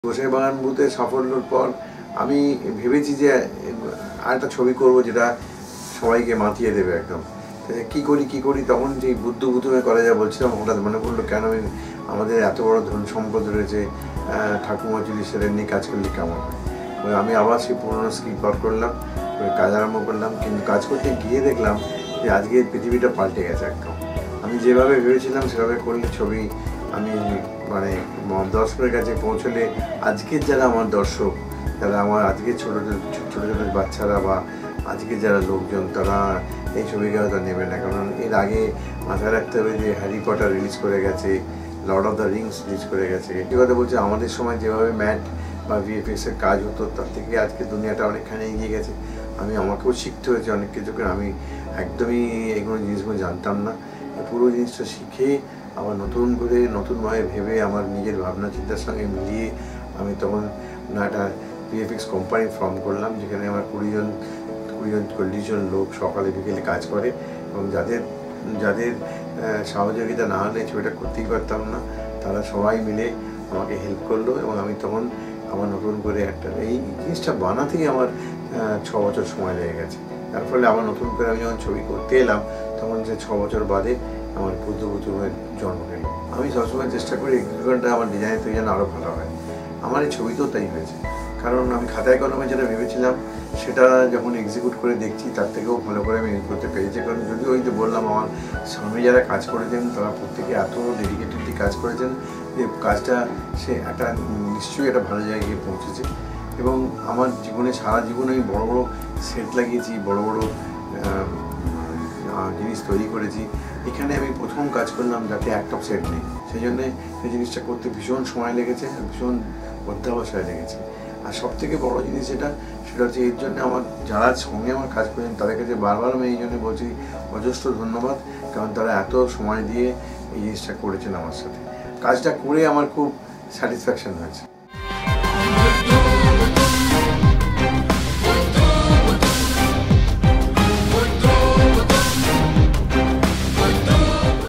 DREAM, PLANNING AND EXECUTION RESULTING IN “BUDDHU BHUTUM” – IN NITISH ROY’S WORDS_(Part-2)
THE DREAM, THE SUBSEQUENT PLANNING AND EXECUTION THAT RESULTS IN “BUDDHU BHUTUM” BEING MADE – AS SAID BY NITISH ROY